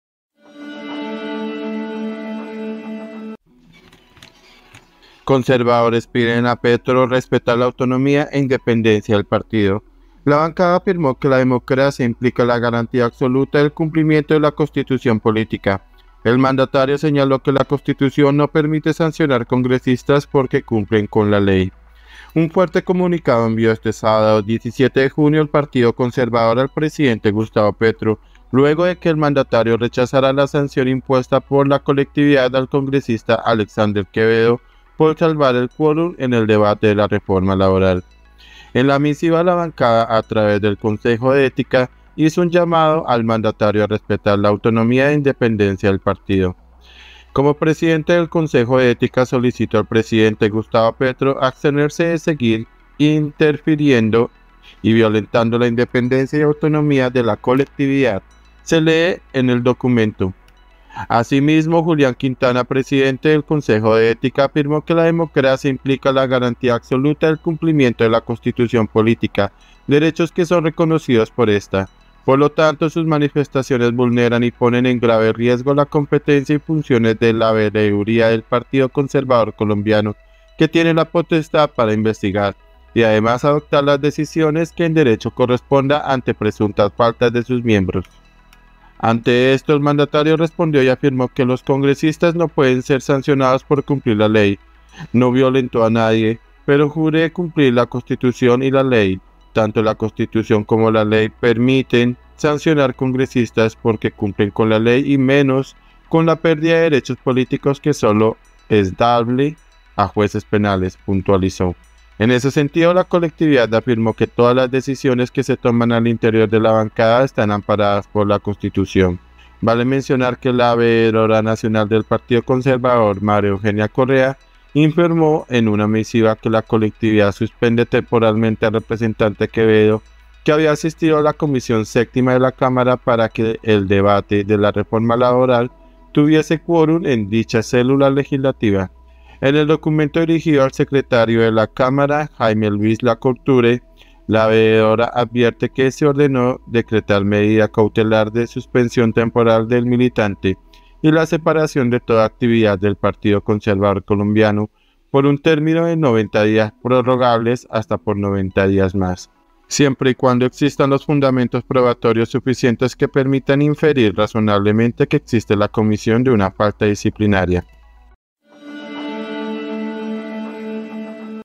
🛑 FUERTE COMUNICADO DEL PARTIDO CONSERVADOR AL PRESIDENTE GUSTAVO PETRO
Un fuerte comunicado envió este sábado 17 de junio el Partido Conservador al presidente Gustavo Petro luego de que el mandatario rechazara la sanción impuesta por la colectividad al congresista Alexander Quevedo por salvar el ‘quorum’ en el debate de la reforma laboral.
Fuente El Heraldo